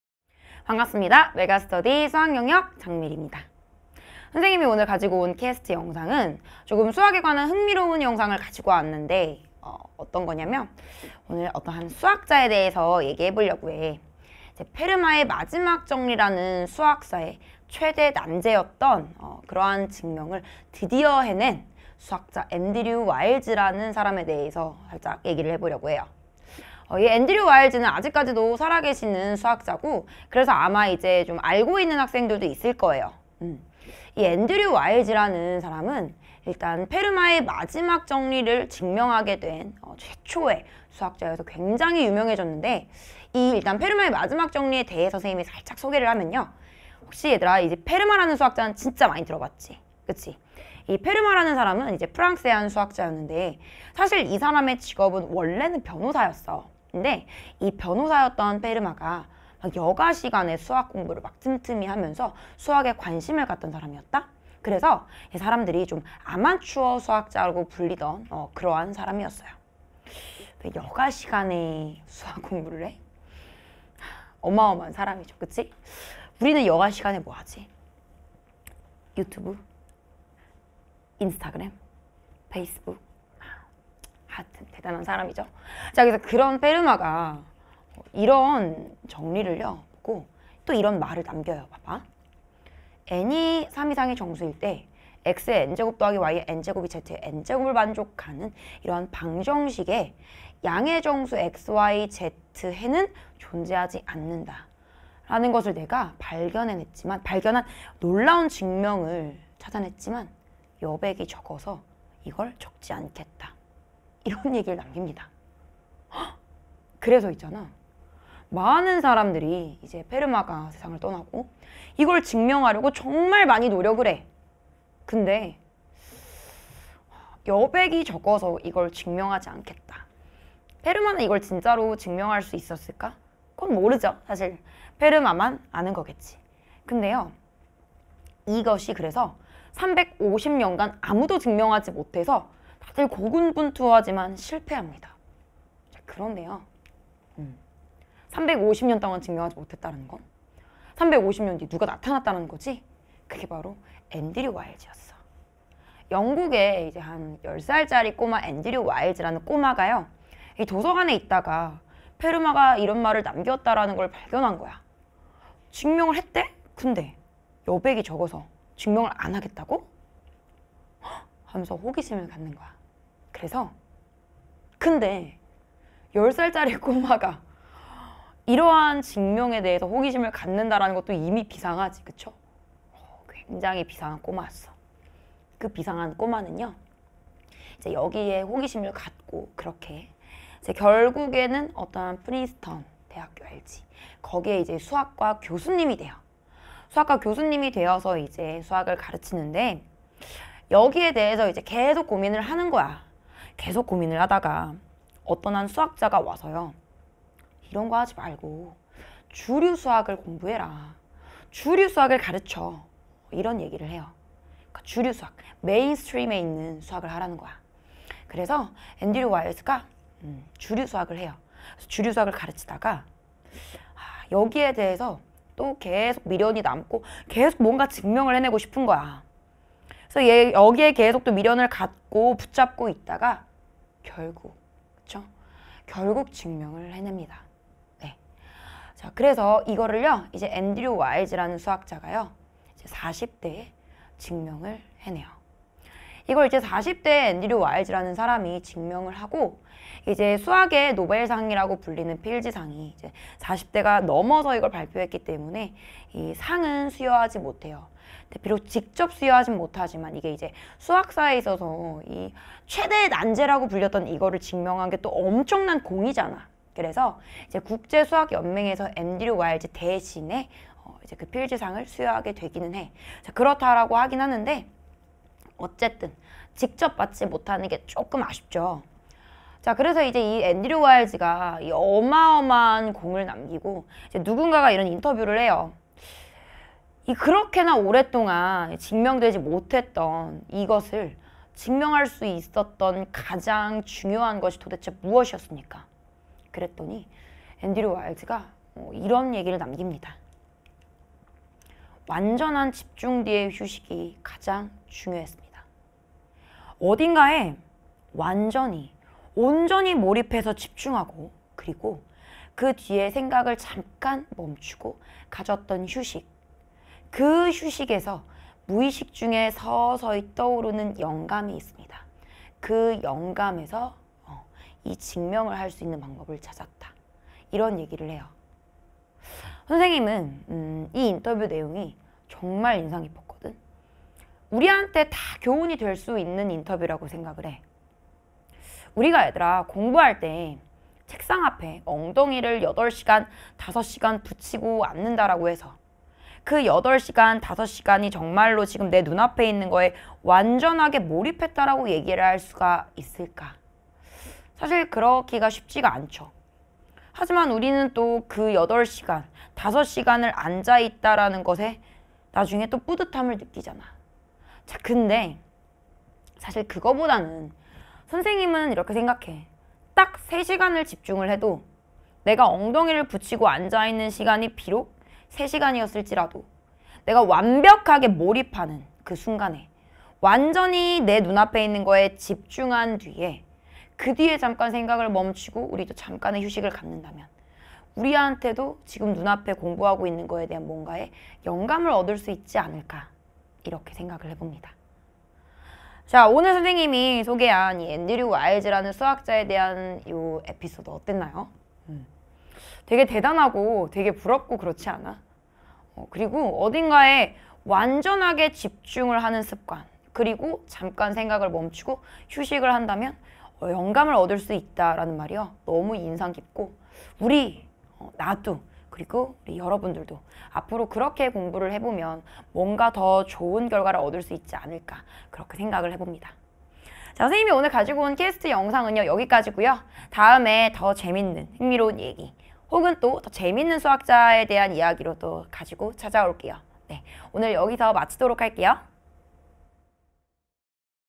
[메가스터디] 수학 장미리 쌤 - [수행평가] 수학자 엔드류 와일즈
수학사 최대의 미스터리, 페르마의 마지막 정리!
그리고 드디어 증명해낸 수학자 엔드류 와일즈!!

오늘 그 흥미진진한 내용을 장미리 선생님이 준비했습니다~
문제풀이 형식 외 수행평가가 있는 학교라면 활용해 볼 수 있겠네요!

이야기에서 얻을 수 있는 교훈도 있다니,
자세한 내용은 영상을 통해 확인해 볼까요?

▶ 장미리 선생님에 대해 더 알고싶다면?